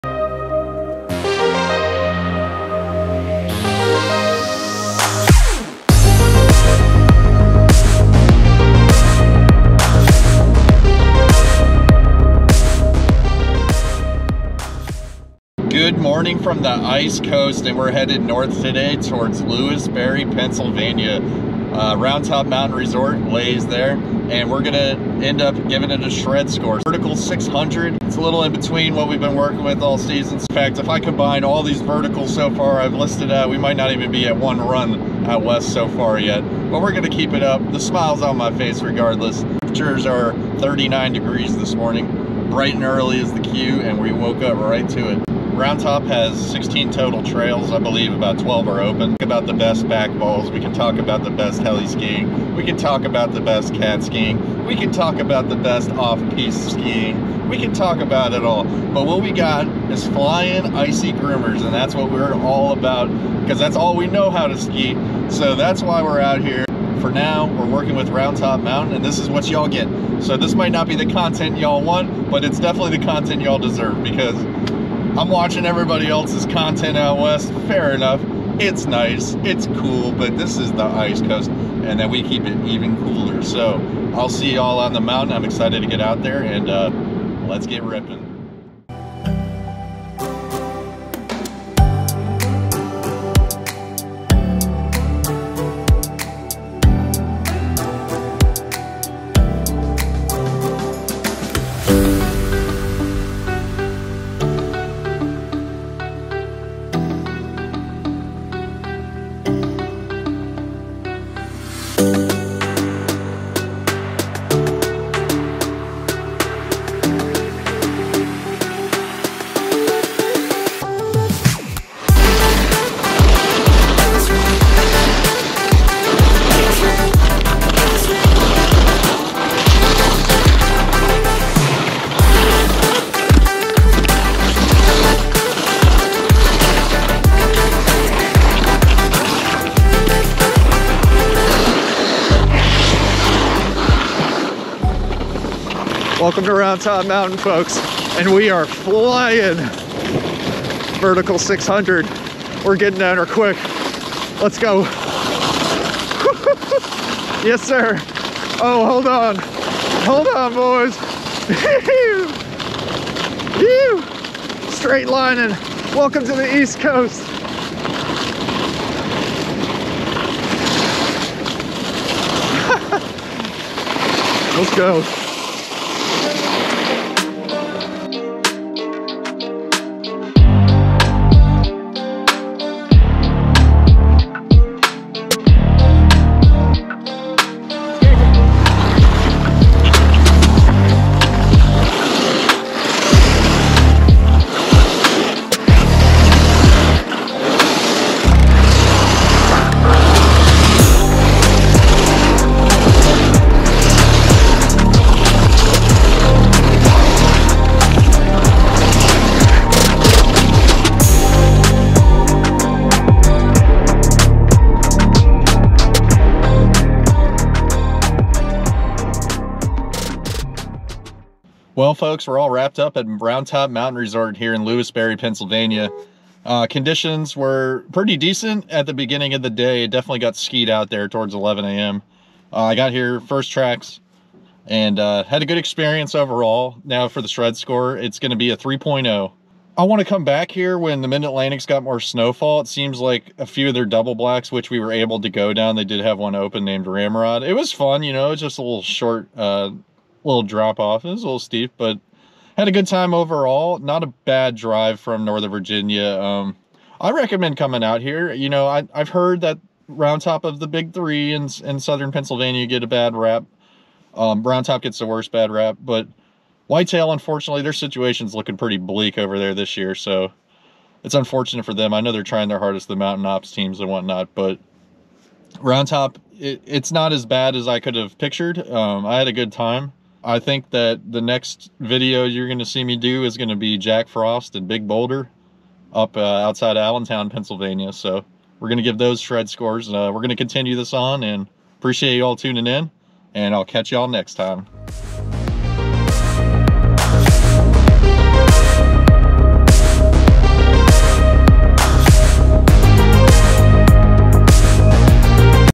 Skiing at Roundtop Mountain Resort, PA
A quick trip to the Keystone State to visit Ski Roundtop Mountain Resort in Lewisberry, PA